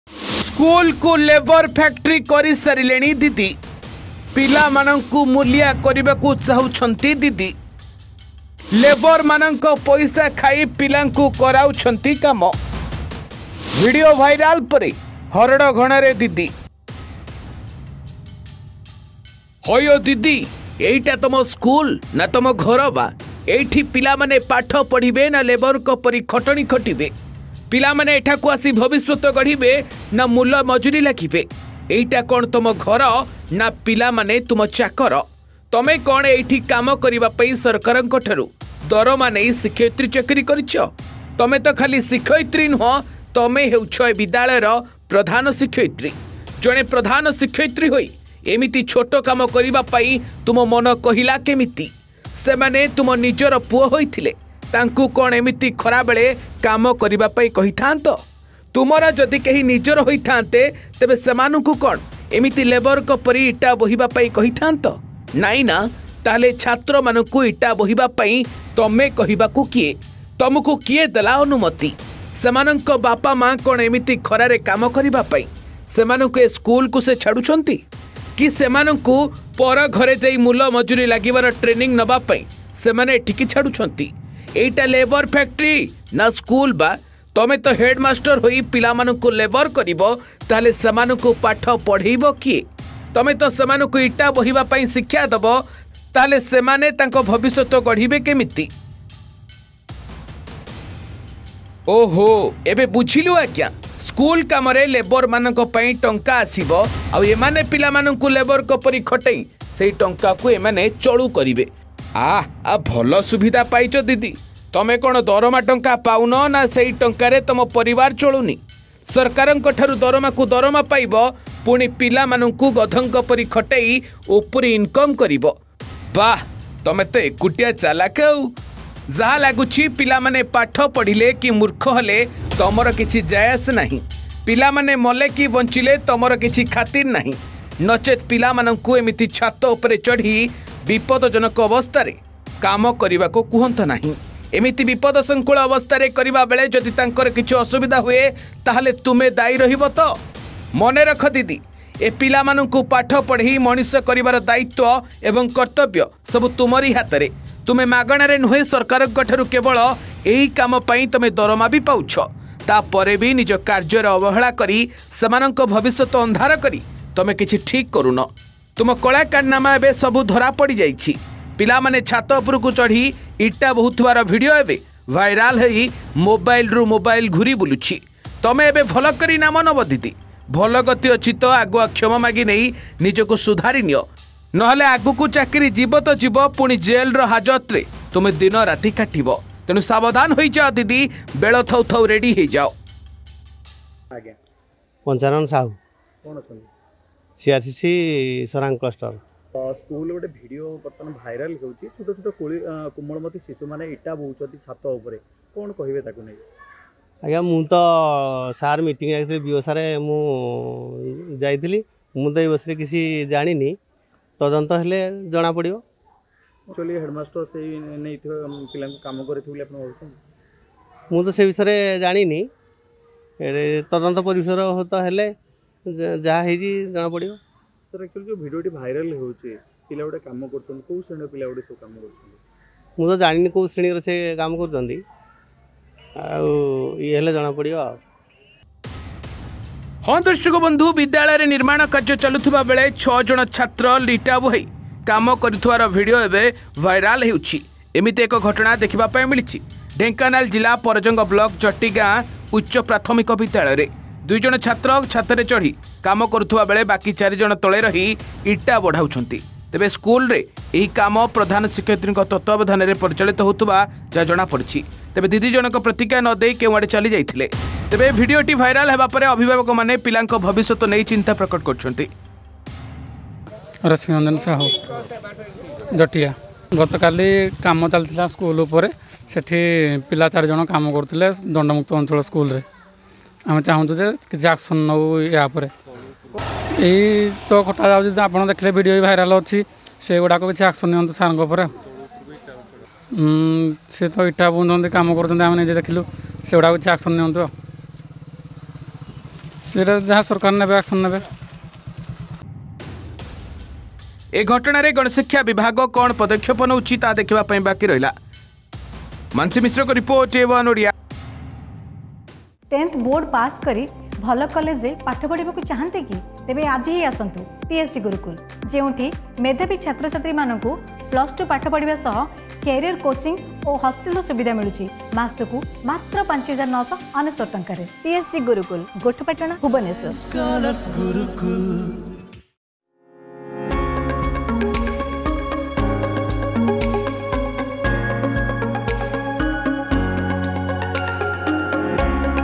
ହଇଓ ଦିଦି ତମେ କଣ ଏ ଛୋଟ ଛୋଟ ପିଲାମାନଙ୍କୁ ତମ ଘରର ମୁଲିଆ ବୋଲି ଭାବୁଛ କି |Teacher Story | Student | A1 ODIA
🔆MORE UDATE🔆